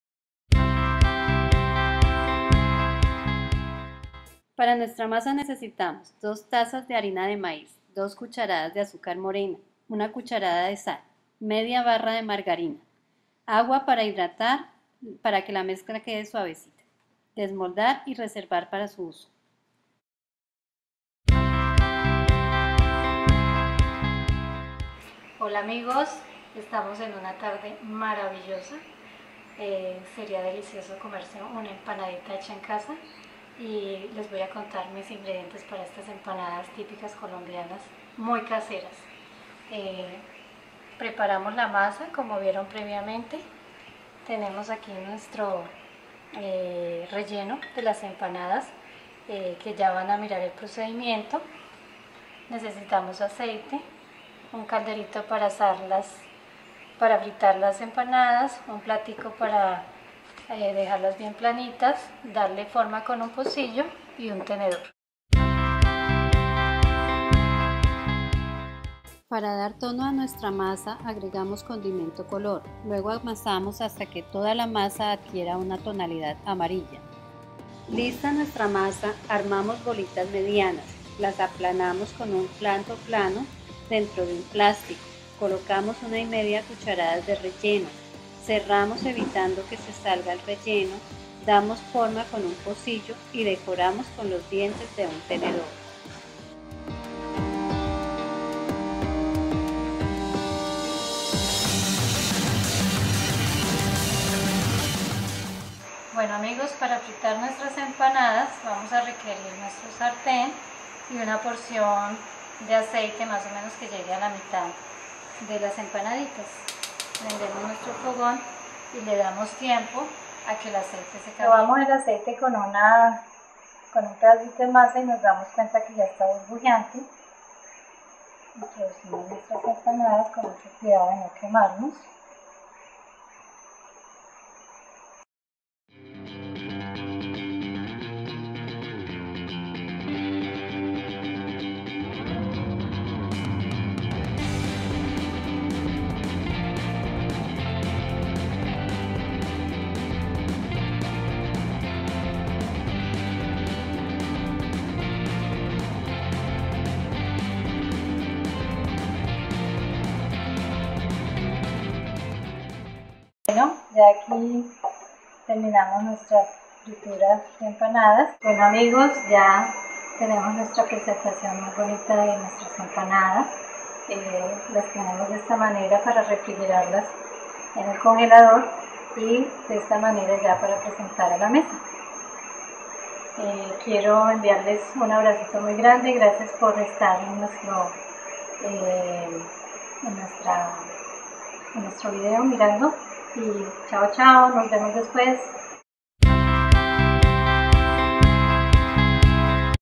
EMPANADAS COLOMBIANAS
Deliciosas empanadas colombianas hechas en casa, con ingredientes comunes en nuestra mesa, las puedes preparar con carne o con pollo y adicionarle el ingrediente de tu preferencia...